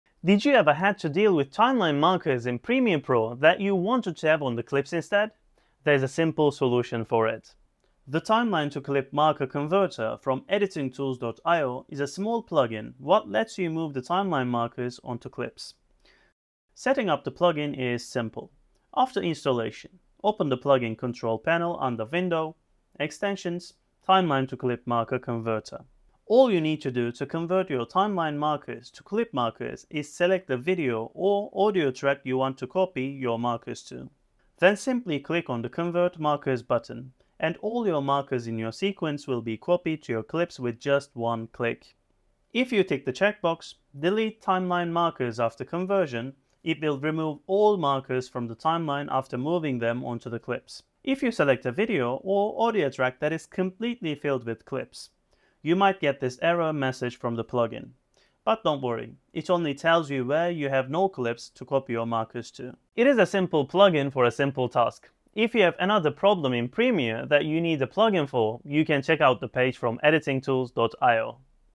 How to move Timeline Markers on to Clips in Premiere Pro - Plugin
This simple Premiere Pro plugin lets you move Timeline Markers in Premiere Pro on to clips: Timeline to Clip Marker Converter

Thanks to @Sone for the Tutorial.